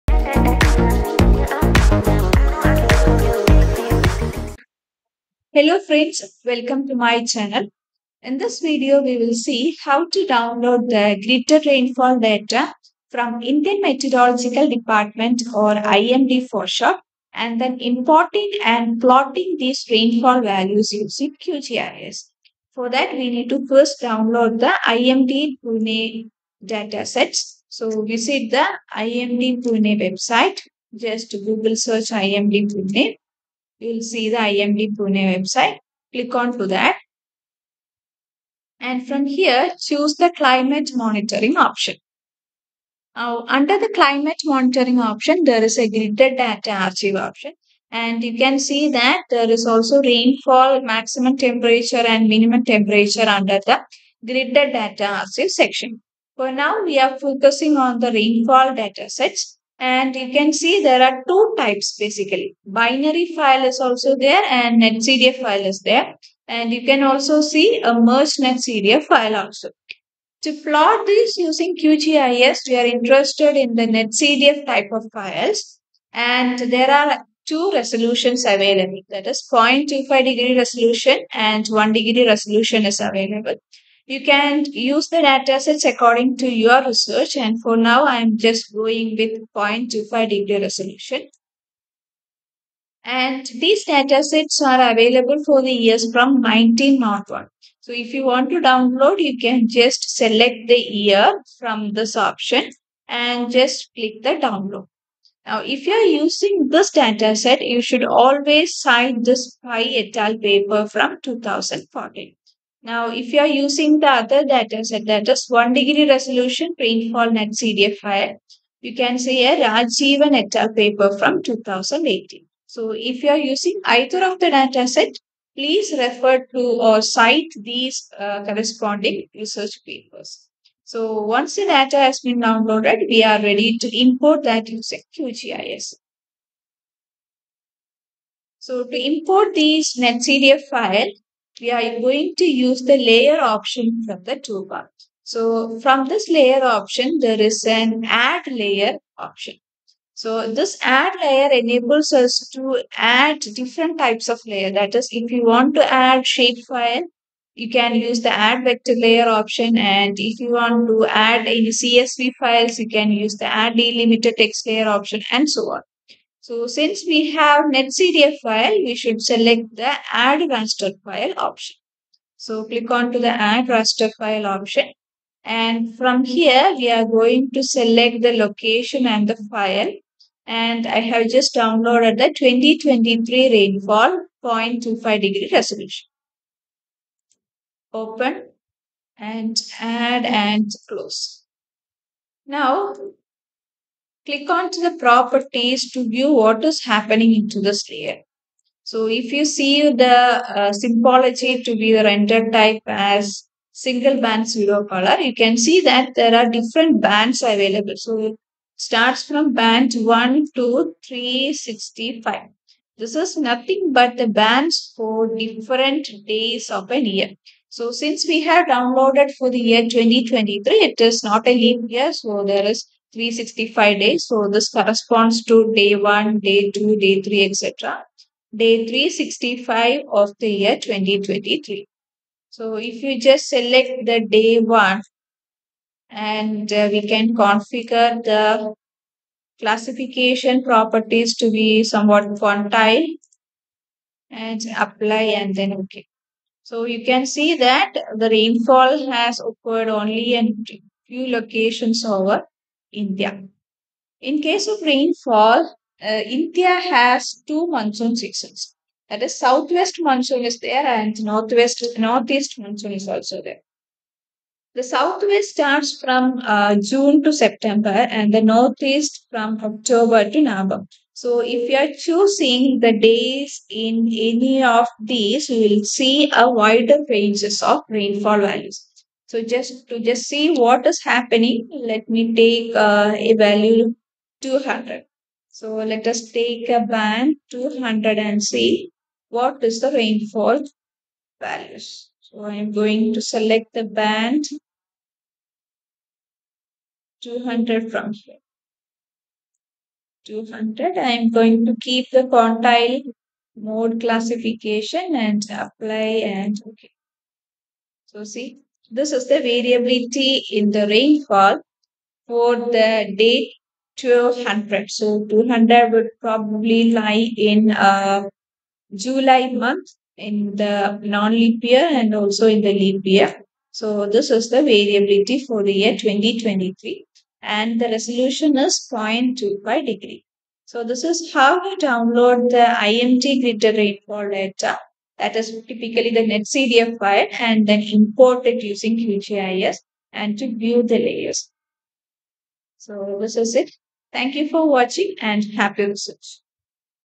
Download gridded rainfall data (NetCDF) from IMD and plotting using QGIS
This video is about how to download the gridded rainfall data from the Indian Meteorological Department website, import the data into QGIS (3.34.0) and plotting the various bands.